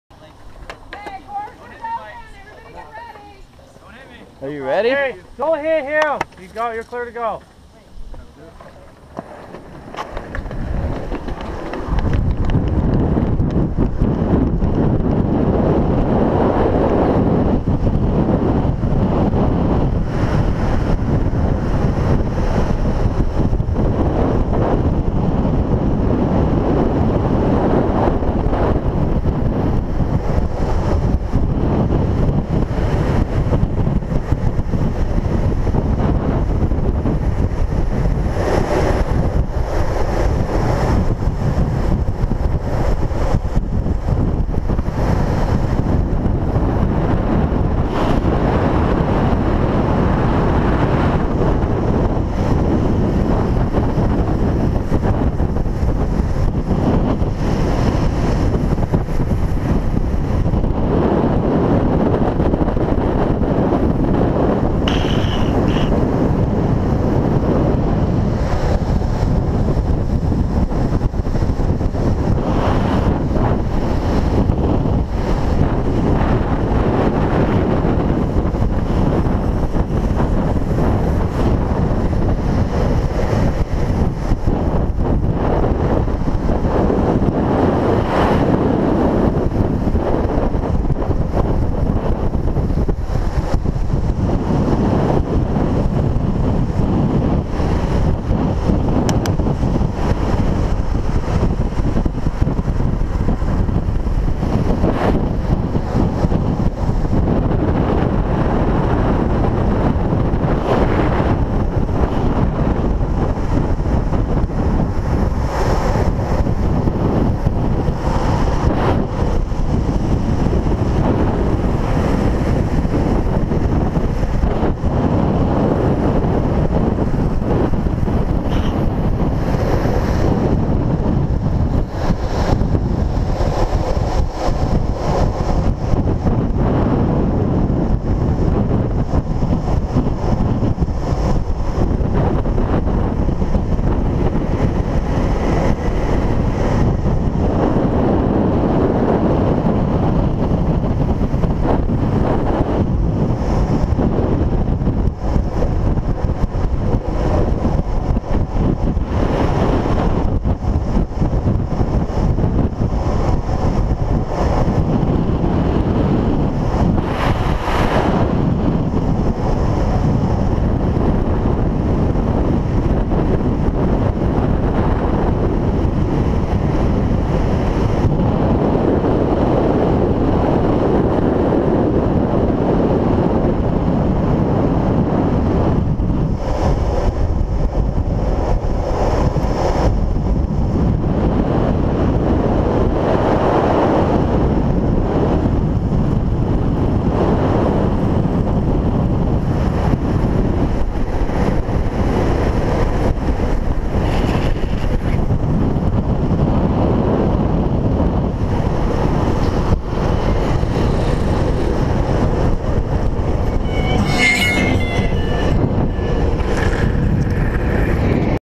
Maryhill FOS 2013 Raw Run
One of my last practice runs at Maryhill this year.